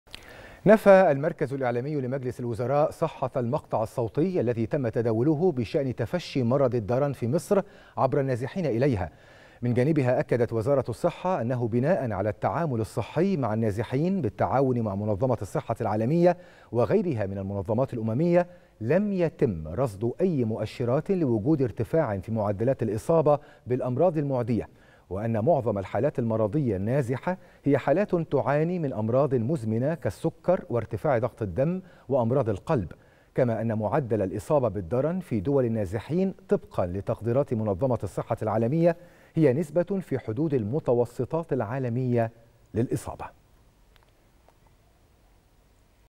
نشرة الثالثة | إعلامي مجلس الوزراء ينفي تفشي مرض الدرن عبر النازحين إلى مصر
إعلامي مجلس الوزراء ينفي تفشي مرض الدرن عبر النازحين إلى مصر